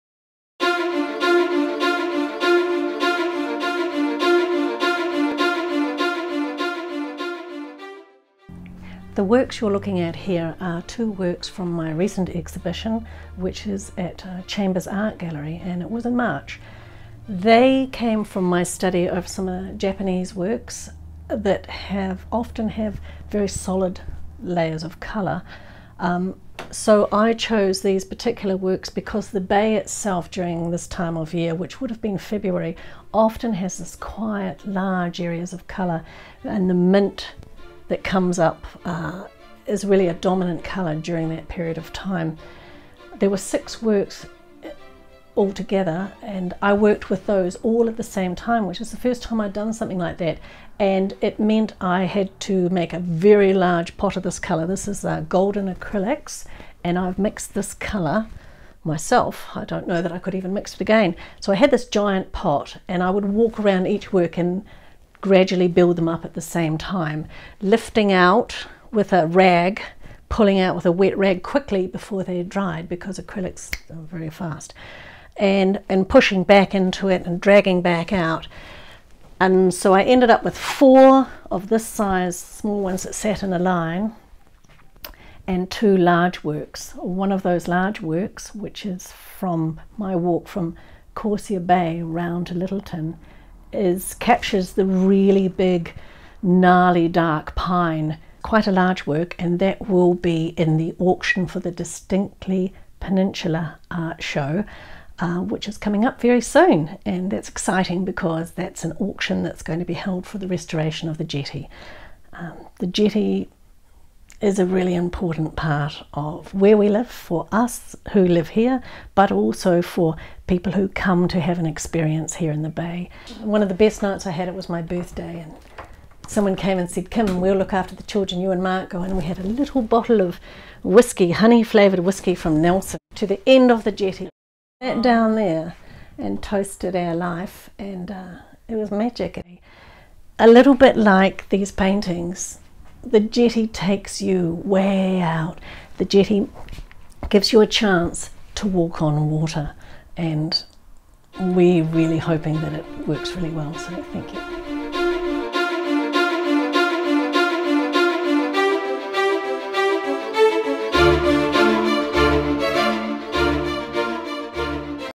Art Auction Distinctly Peninsula - Fundraiser for the Governors Bay Jetty - Meet artist Kim Hennessy
The Distinctly Peninsula Art Auction is a fundraiser for the rebuild of the historic, 300m long Governors Bay Jetty. All art work in the auction will be displayed in an exhibition at the Tannery in Woolston from 18 - 22 October 2022.
Join us on Saturday, 29th October, 3pm at the Otomiro Hotel (formerly Governors Bay Hotel) in Governors Bay for the Art Auction!